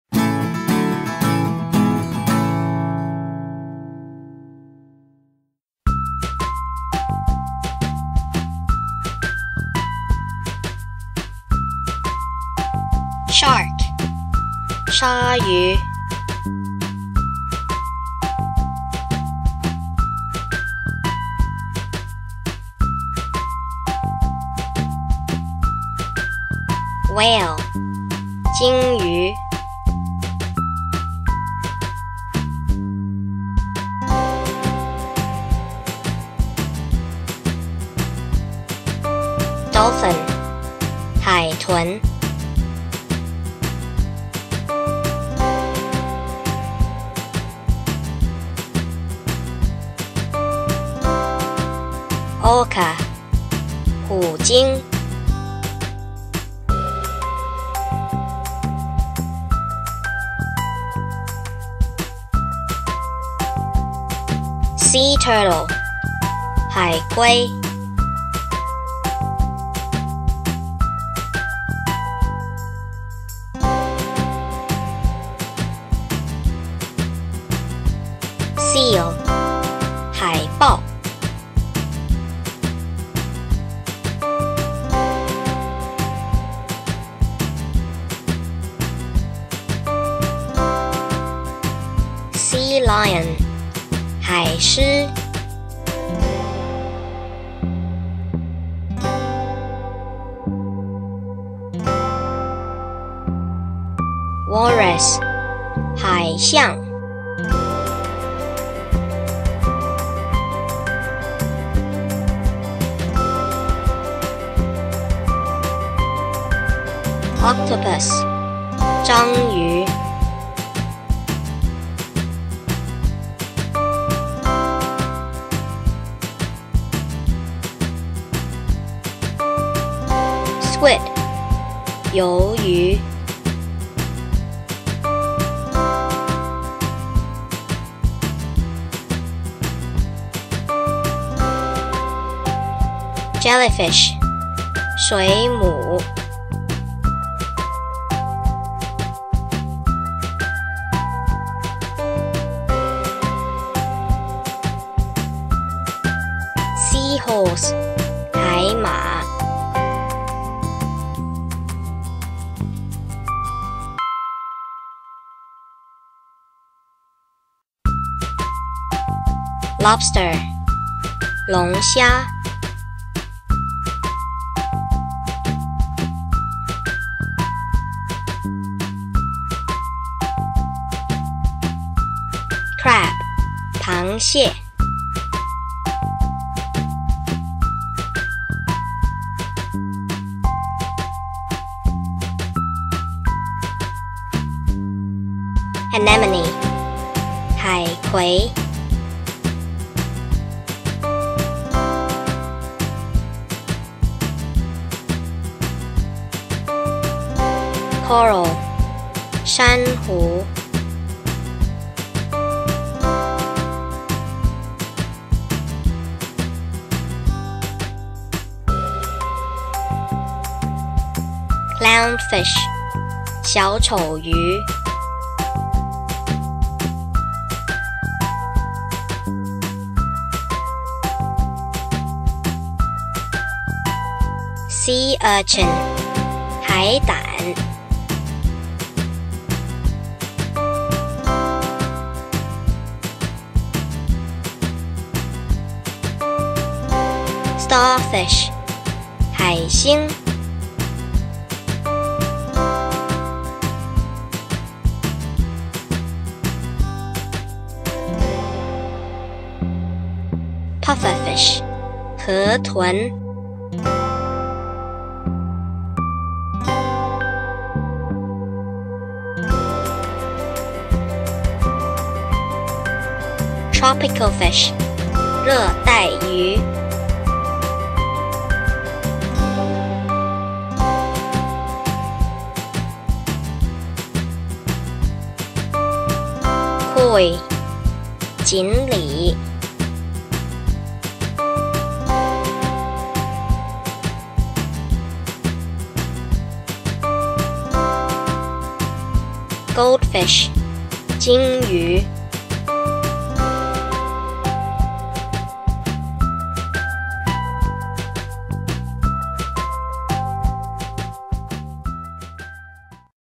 Learn Mandarin for Kids | Learn Animal Names in Mandarin with Animated Drawings
"It's Mandarin Learning Time! Kids, Let's learn some sea animal names in mandarin today!